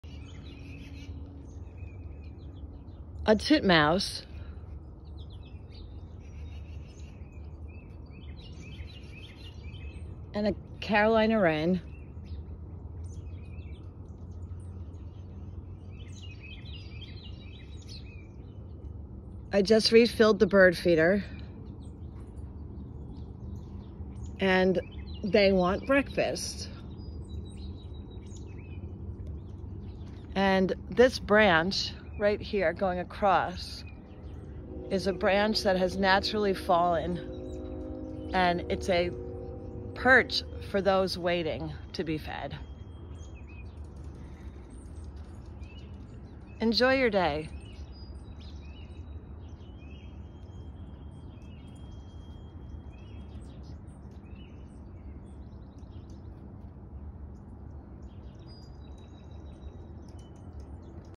Sunday Morning Nature
This morning I went out to refill the birdfeeder and love to listen to the birds that I hear. A titmouse that I thought was a black capped chickadee. You learn something new every day. Thanks to my Merlin ID app.